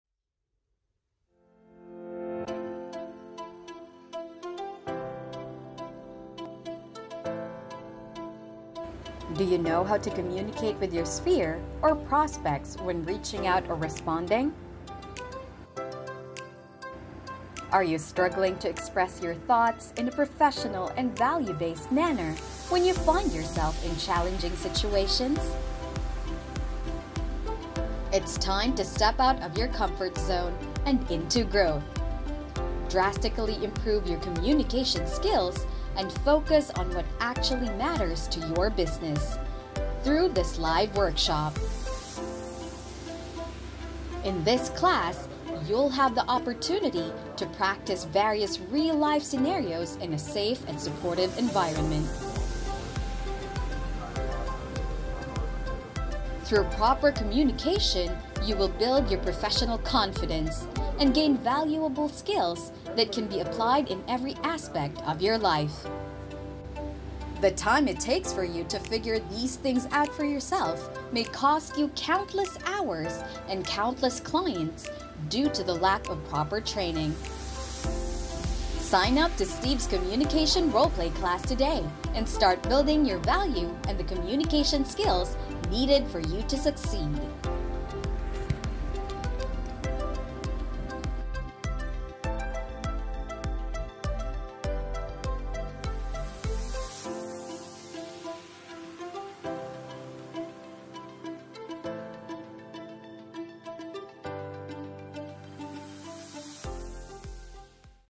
Revolutionize Your Business with These Proven Communication Techniques!
Don't let poor communication skills hold you back any longer! Join Steve's Communication Role-Play Class and learn how to effectively communicate with your sphere or prospects in any situation. Gain the confidence you need to excel in job interviews, conflict resolution, public speaking, and more. Don't miss out on this opportunity to improve your professional value and take your business to the next level.